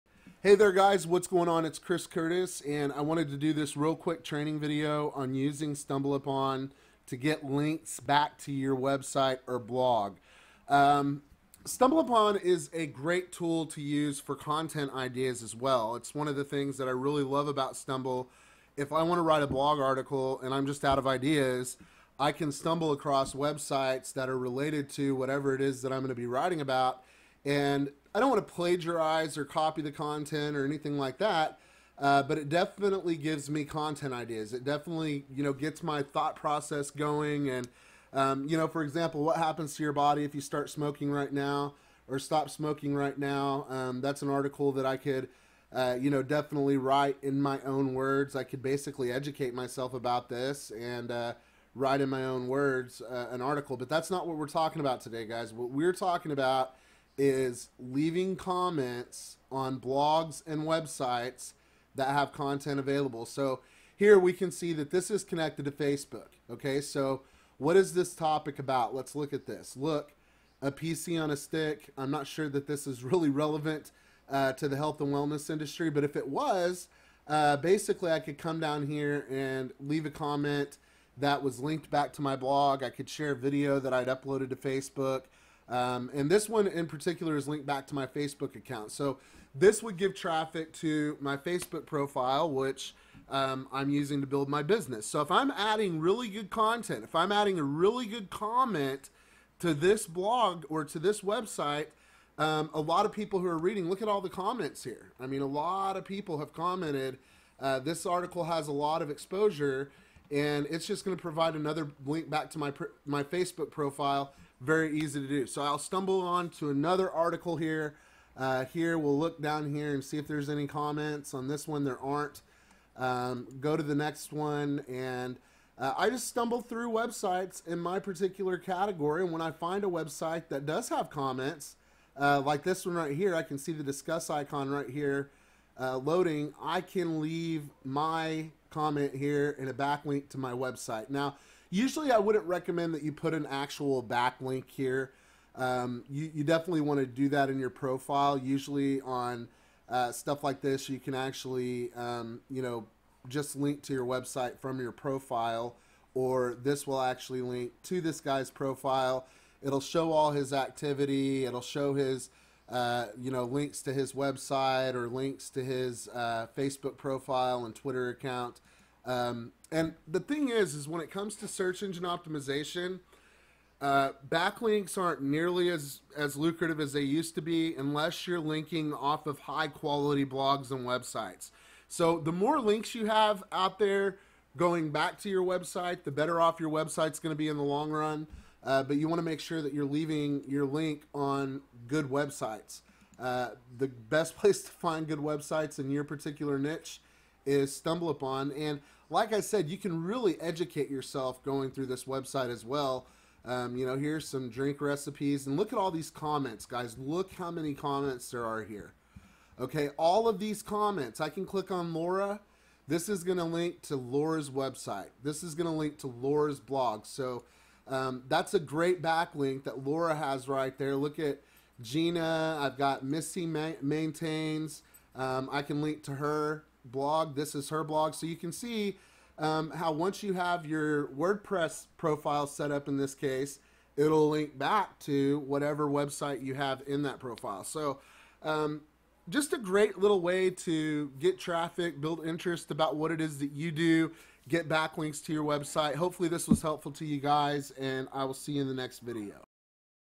Internet Traffic Strategy - Stumble Upon for backlinks
This is a great way to build backlinks and get interest in whatever it is that you are doing.